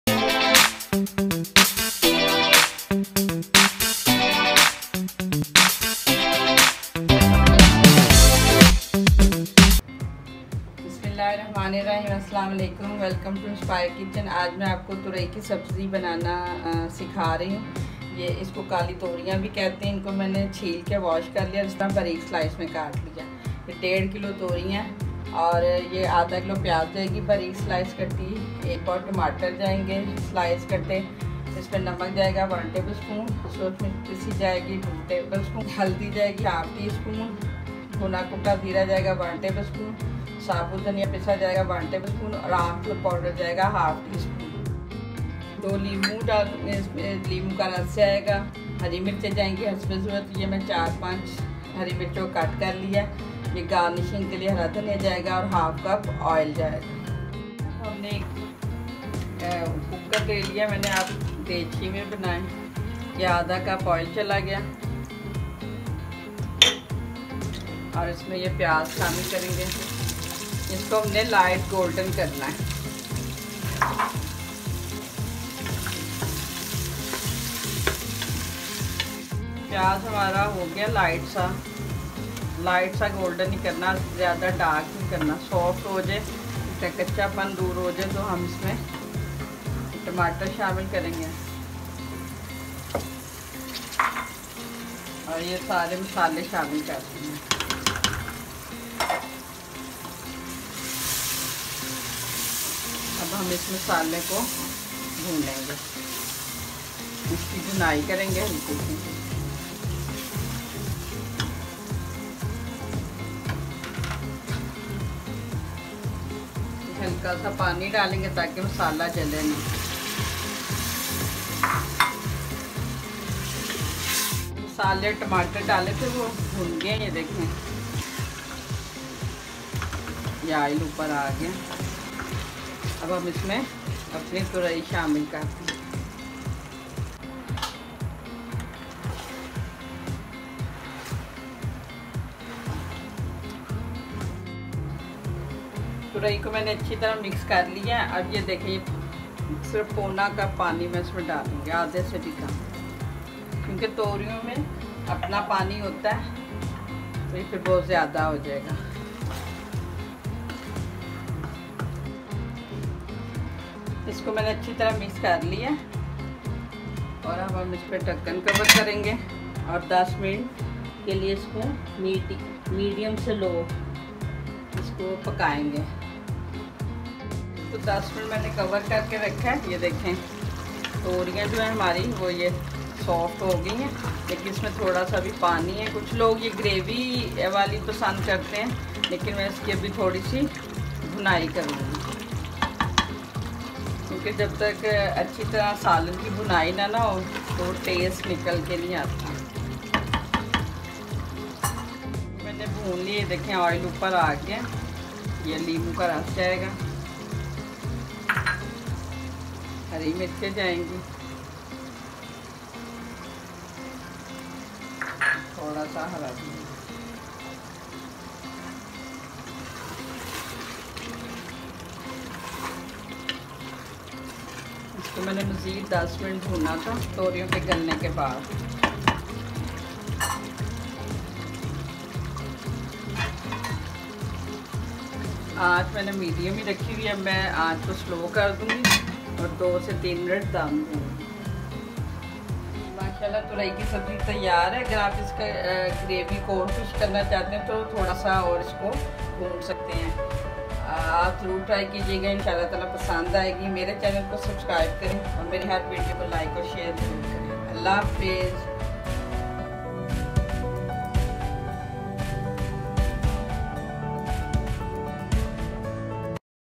Tori ki Sabzi | Easy & Quick Recipe| توری کی سبزی | By Inspire Kitchen
This recipe is really easy to make at home especially for beginners. You can get a well tasted vegetable with basic masalas.

Also give me your feedback,as it’ll be supportive to me. I can bring more interesting and tasteful recipes for you.

Thanks 🙏🏻